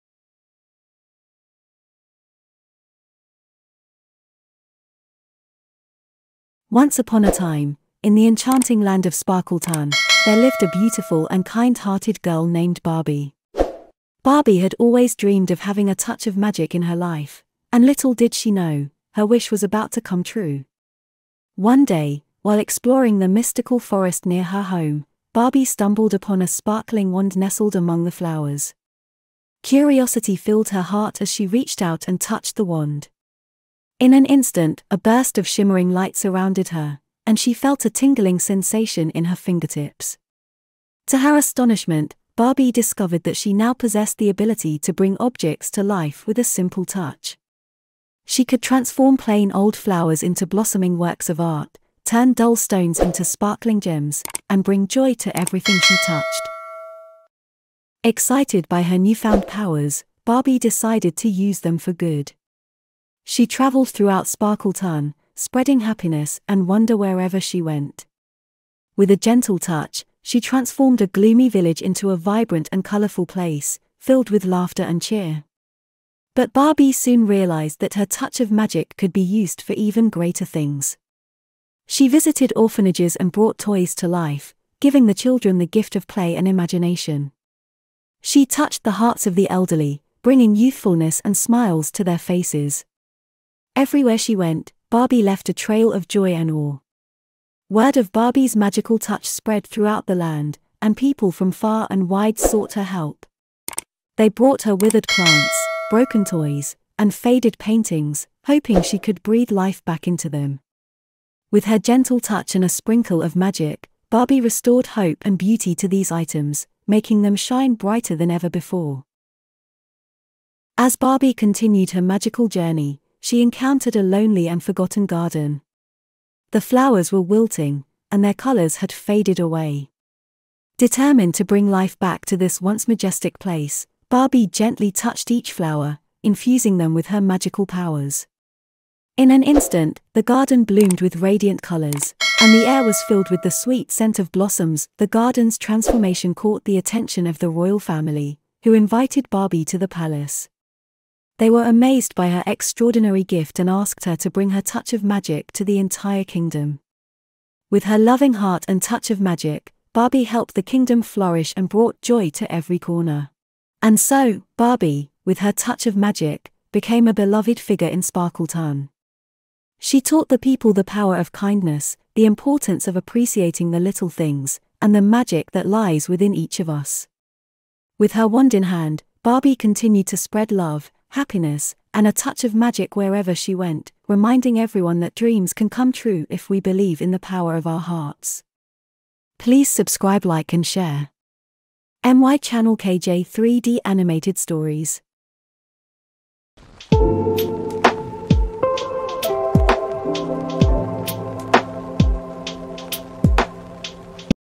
"Barbie: A Touch of Magic"💖💖💖✨✨💖✨💖
💖✨✨✨💖💞💞✨💖"Barbie: A Touch of Magic" is an enchanting and heartwarming story that follows the extraordinary journey of Barbie, a young girl who discovers a magical wand that grants her the power to bring objects to life with a simple touch. With her newfound abilities, Barbie embarks on a mission to spread joy, beauty, and wonder throughout the land of Sparkleton. This captivating tale celebrates the transformative power of kindness, imagination, and the belief in the extraordinary within ourselves. Join Barbie as she uses her touch of magic to bring happiness to those she encounters, reminding readers that a little bit of magic can go a long way in brightening the world around us.💖💖✨✨💖💞✨💖💖